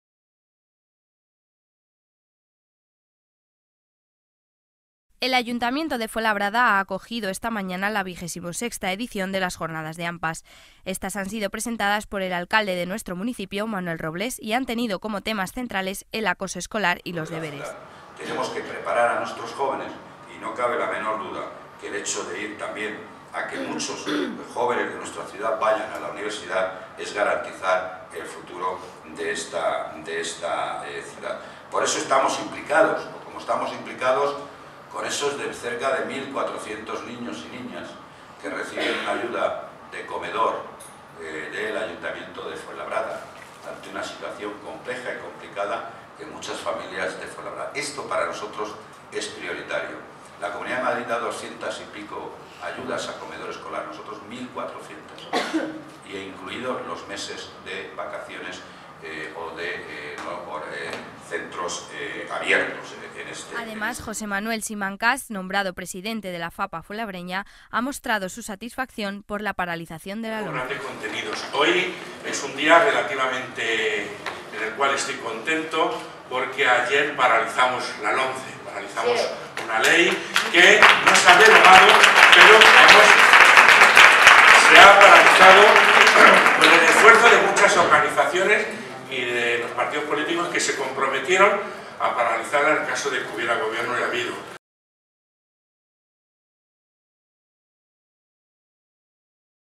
Presentación de las XXVI Jornadas de AMPAS
El acoso escolar y los deberes son los dos asuntos sobre los que se debatirá en las XXVI Jornadas de AMPAS que se han celebrado este miércoles en el Salón de Actos del Ayuntamiento.

La Asociación ha elegido para esta edición dos cuestiones de candente actualidad que preocupa mucho a las madres y padres y es necesario debatir de la mano de profesionales, explica el concejal de Educación, Isidoro Ortega.

Las Jornadas se han desarrollado durante todo el día y se iniciado con la charla sobre Modelo educativo con todo para tod@s a cargo del presidente de la Confederación de Asociaciones de Madres y Padres de Alumnos (CEAPA), José Luis Pazos.

El Programa Integral de Seguridad Escolar y el Plan de Acción contra la Violencia Escolar se explicará a todos los asistentes de la mano del Jefe de la Policía Local, Francisco Cano y la coordinadora del Servicio de Asistencia a la Víctima, Yolanda Pecharromán.

Por la tarde el debate ha girado en torno a los deberes escolares, un asunto que tiene muy dividida a la comunidad escolar y sobre el que hablará Miguel Martínez López, autor del libro El mito de los deberes.

El alcalde, Manuel Robles, que ha inaugurado las Jornadas, ha destacado la importancia de celebrar estos encuentros entre profesionales con padres y madres para debatir el modelo educativo y ha recordado la importante apuesta que hace el equipo de Gobierno ya que Fuenlabrada cuenta con la mayor cobertura de ayudas escolares posibles y que este año se ha ampliado a la etapa universitaria.

En estas jornadas además se ha presentado de manera oficial el nuevo presidente de la delegación de la Fapa en Fuenlabrada, José Manuel Simancas.